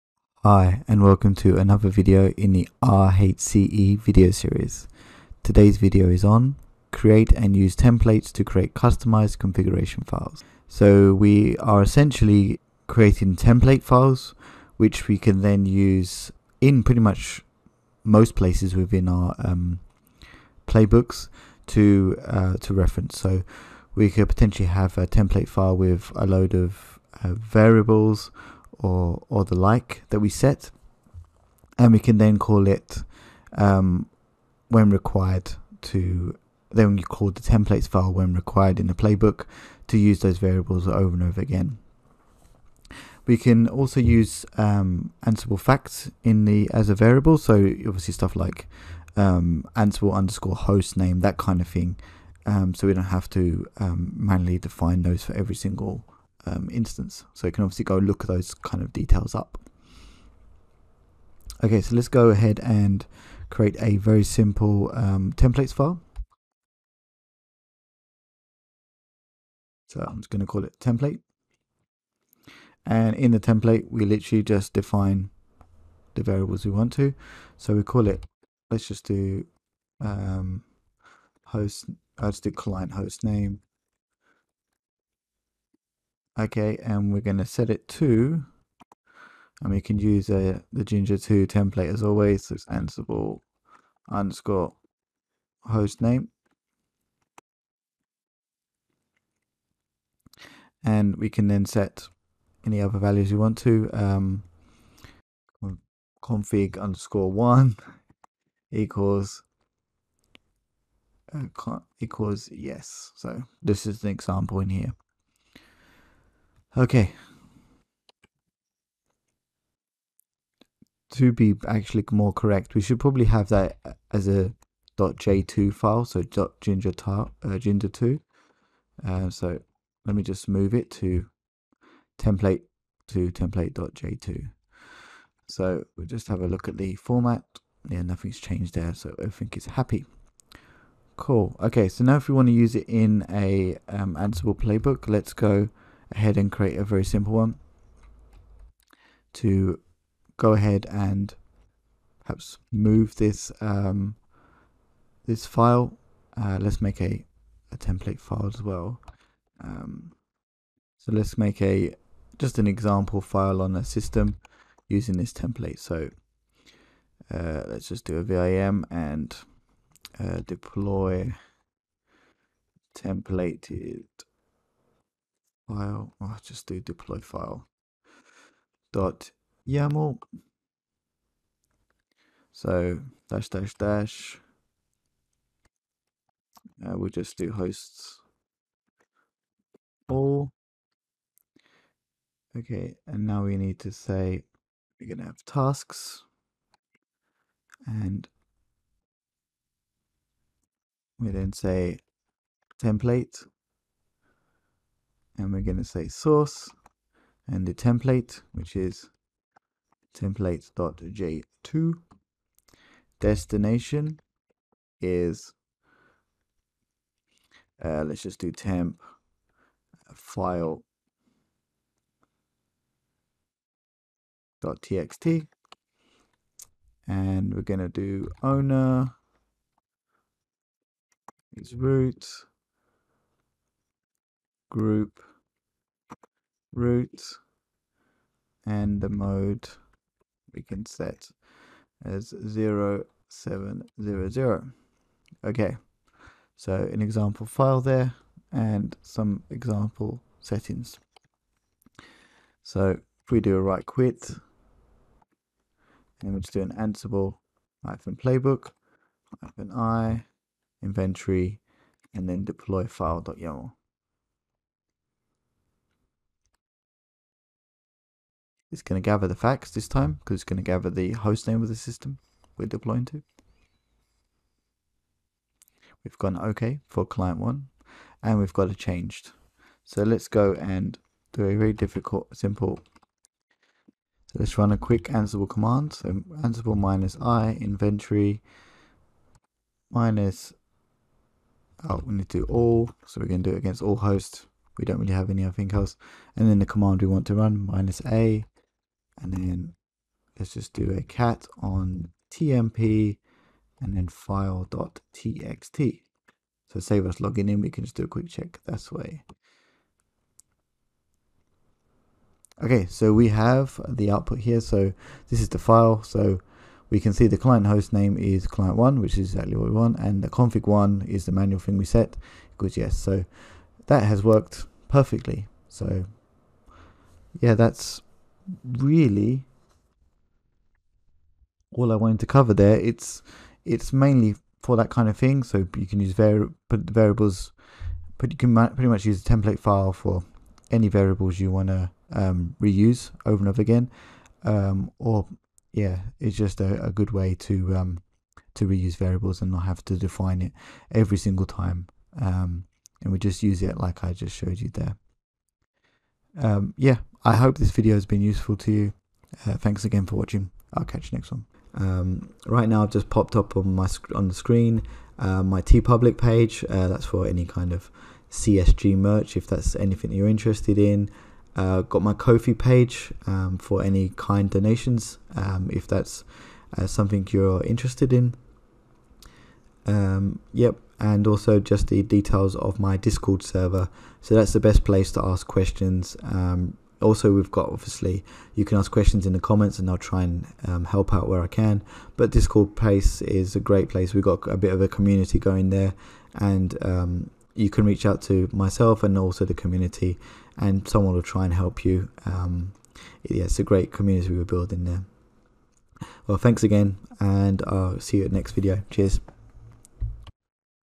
RHCE RHEL 8 - Create and use templates to create customized configuration files
This video is based on RHEL 8.

Video to cover the section 'Create and use templates to create customized configuration files' for the RHCE (Red Hat Certified Engineer).